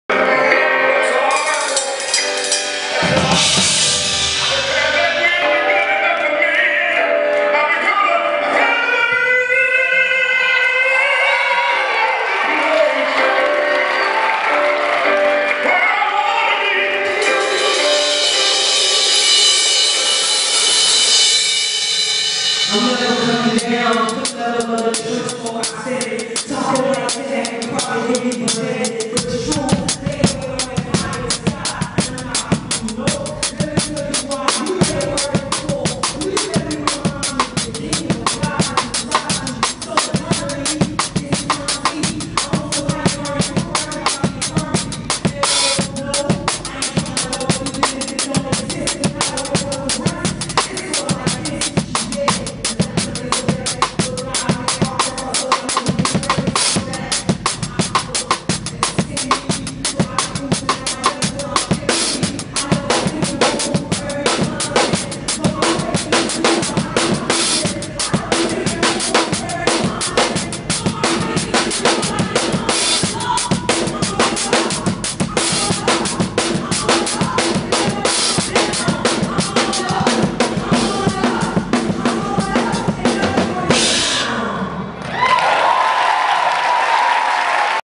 Nicholas Reed II (drummer)
This is the 15 y.o. drummer for ShySpeaks. An incredible guy and wonderful musician. Remember his name NICK REED.